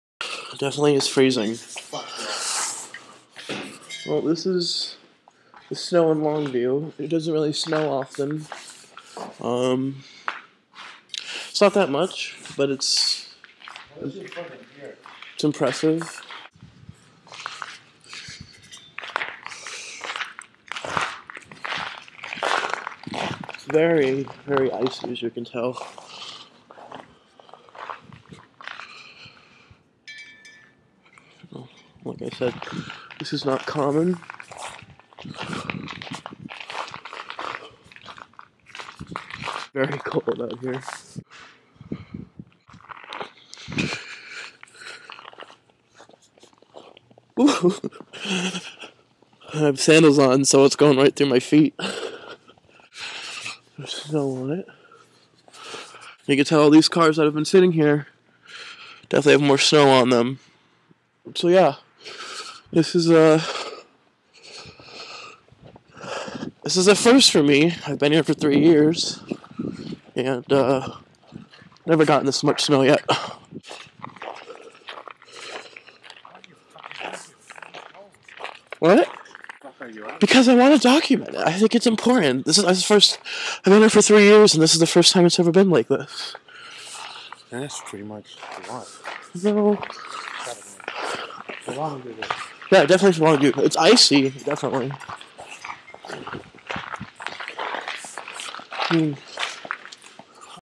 Snowy in Washington!
The most snow we've had in a while.  It's only 2-3 inches I believe, but.. that's uncommon here.